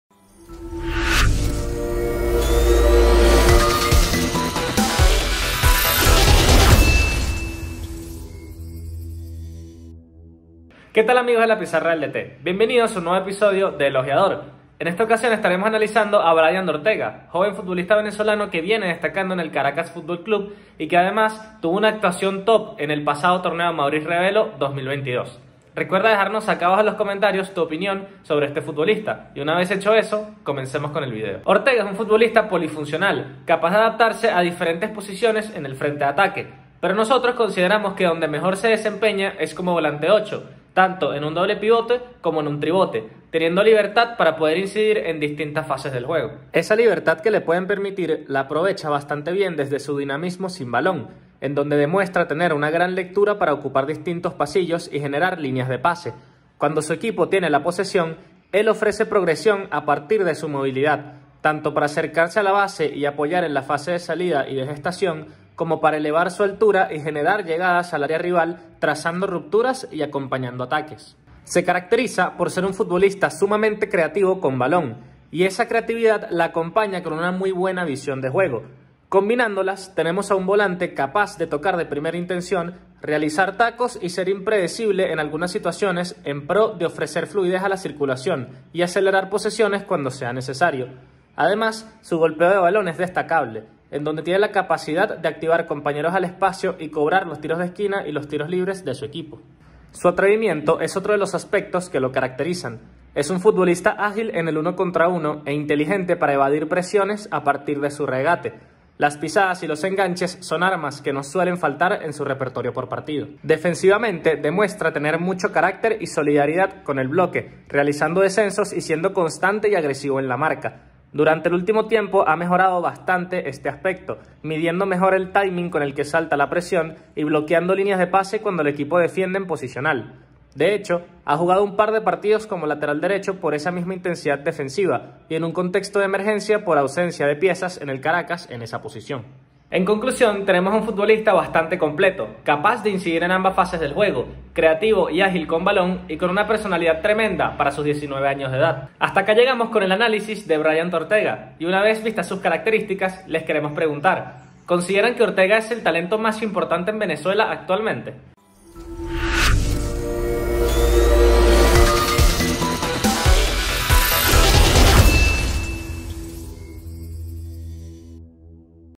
El Ojeador: Bryant Ortega (2003) I Caracas Fútbol Club
¿Qué tal amigos de La Pizarra del DT? Bienvenidos a un nuevo episodio de El Ojeador. En esta ocasión analizamos a Bryant Ortega, joven promesa del Caracas Fútbol Club. Recuerda compartir y dejar tu like si te gustó el contenido. Nos vemos en un próximo video.